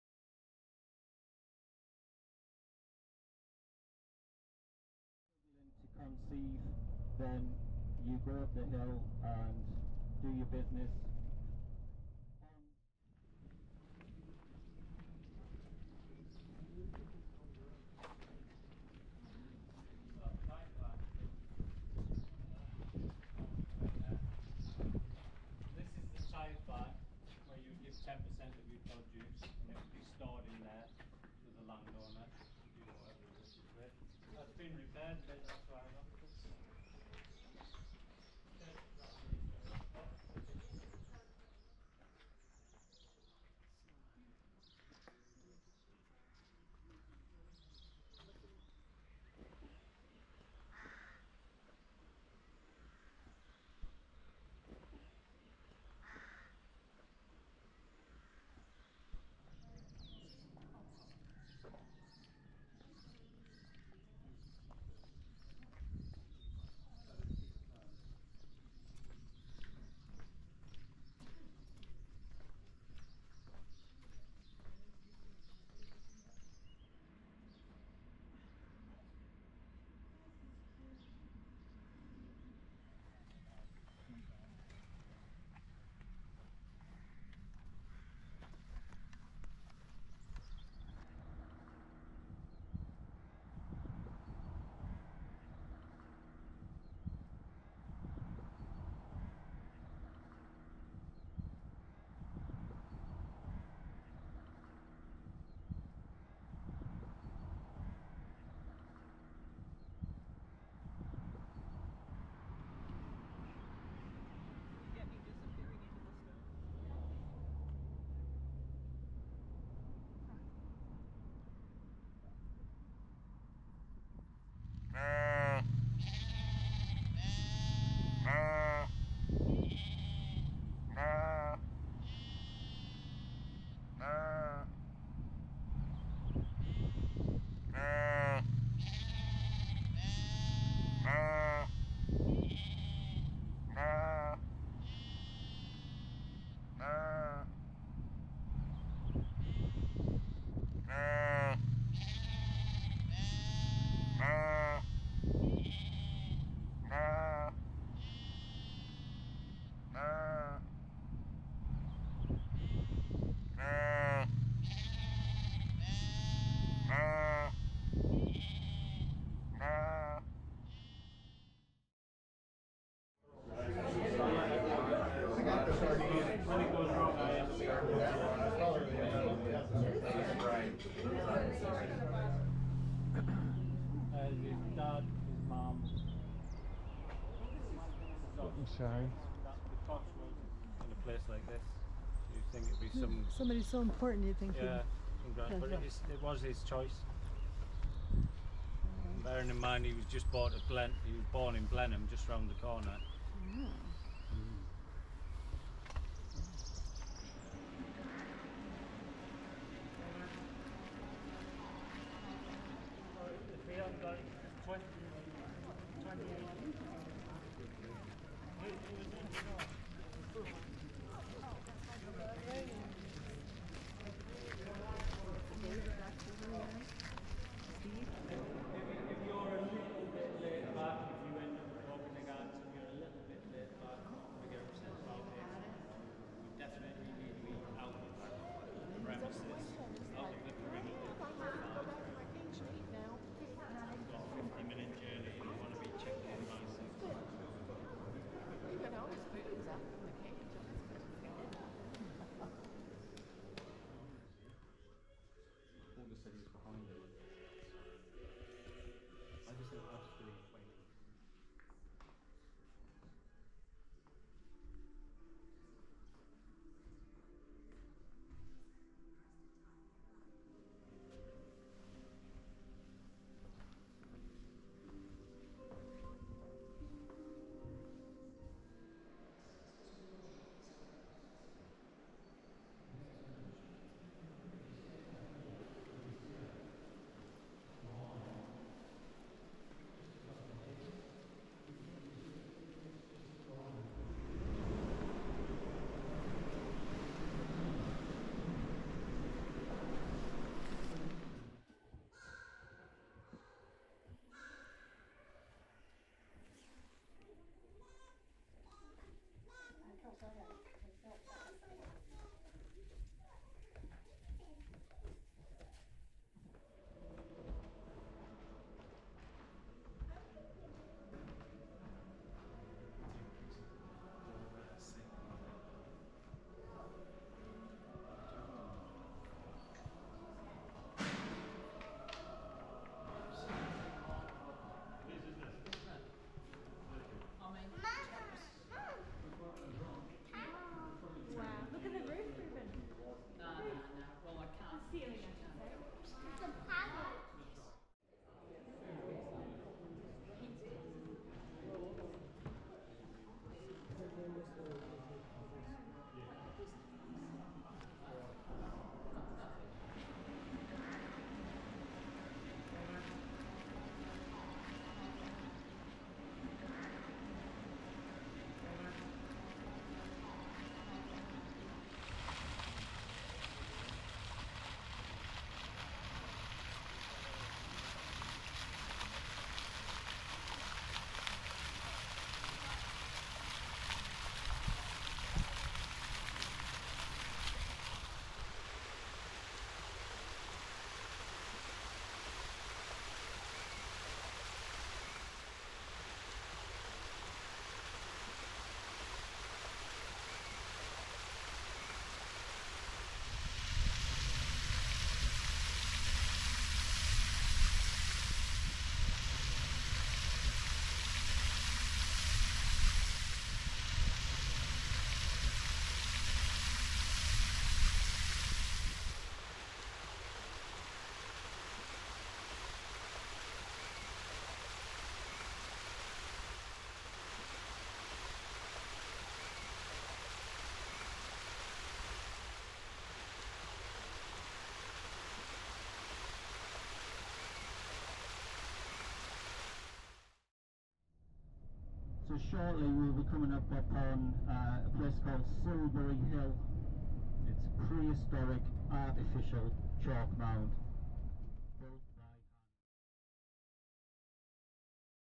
Avebury and Blenheim Palace
Rick Steves Best of England Tour, May 19, 2023. Traveling from Bath, to Avebury, with a brief stop at Lacock Village. After lunch, a visit to Churchill's grave, then a tour of Blenheim Palace.